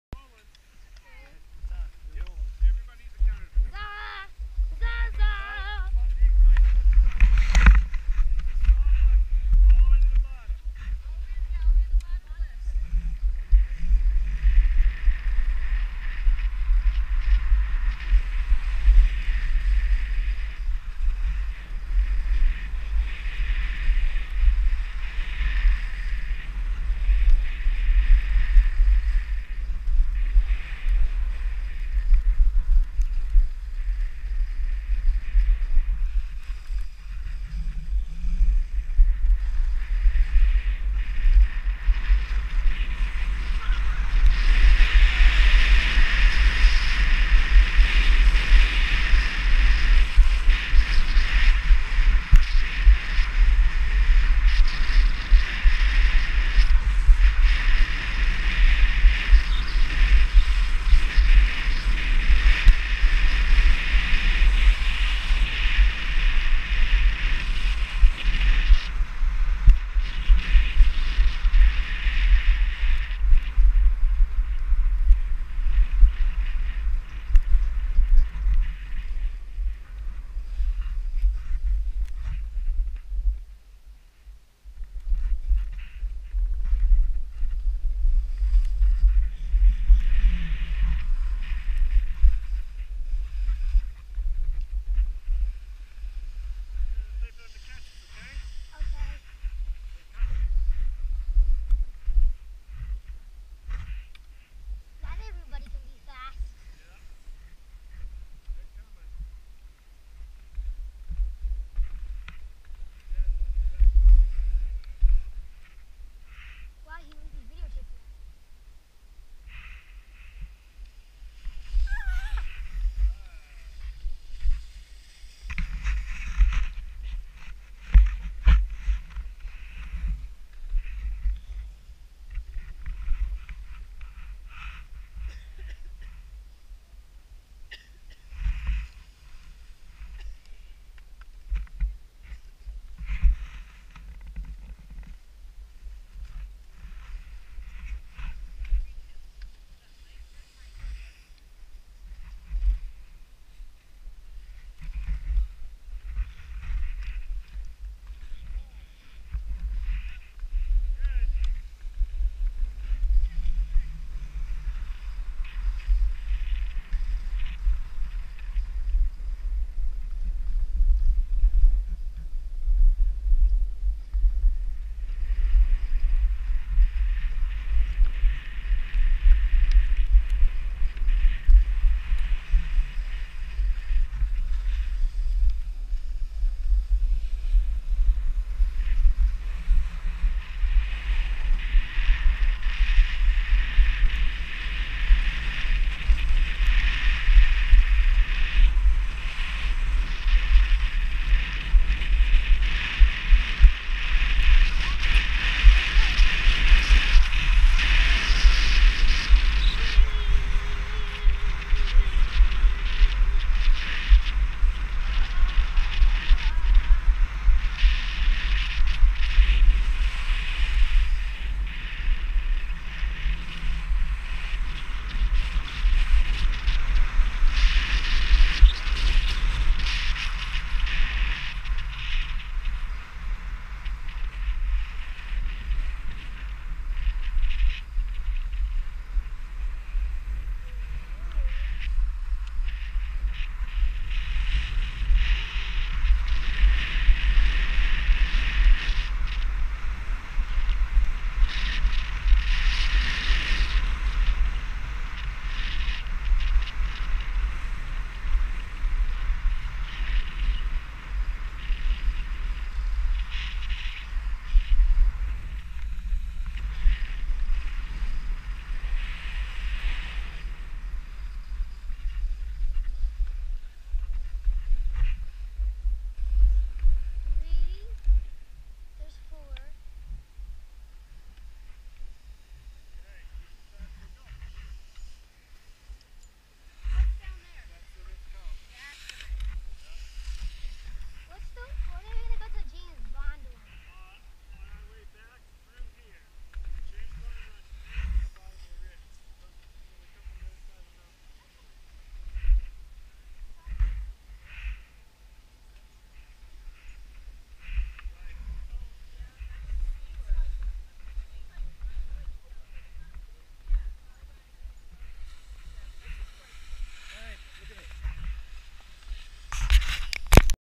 Spring Break 2018
Ryan wearing GoPro in Beaver Creek